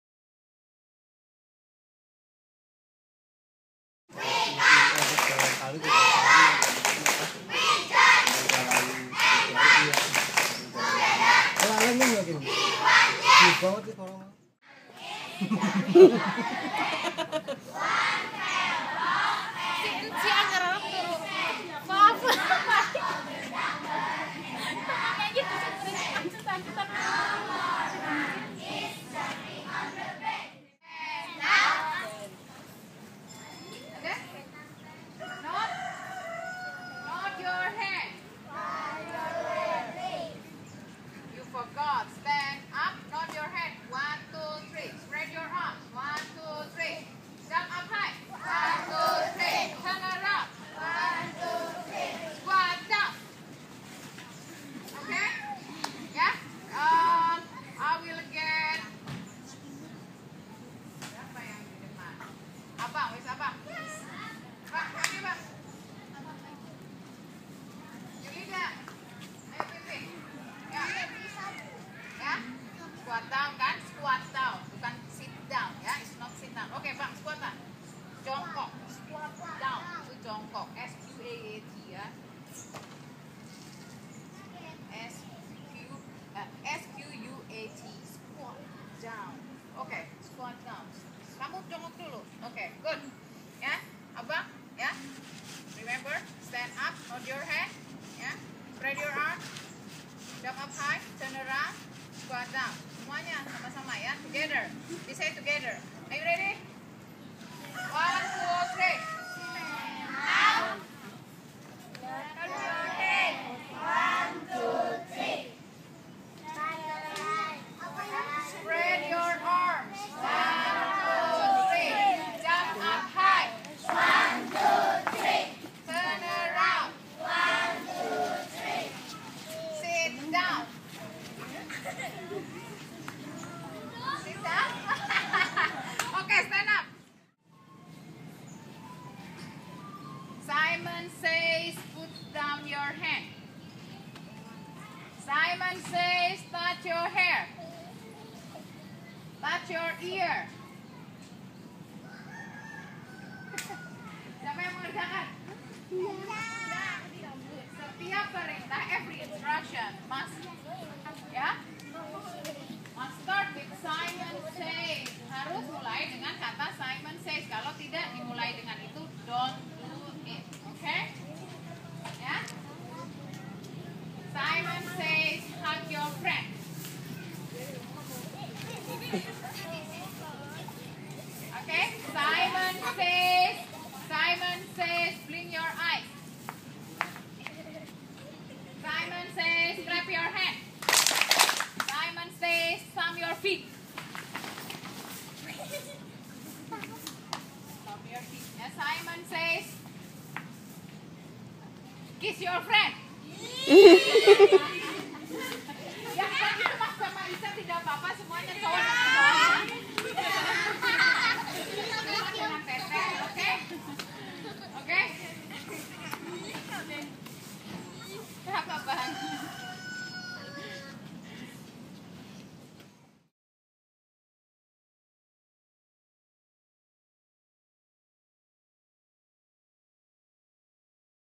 "Simon Says"
Samen met de MB kids maken we een uitstapje. Terwijl we wachten op ons vervoer houdt juf Tia de kids bezig met engelse les en spelletjes.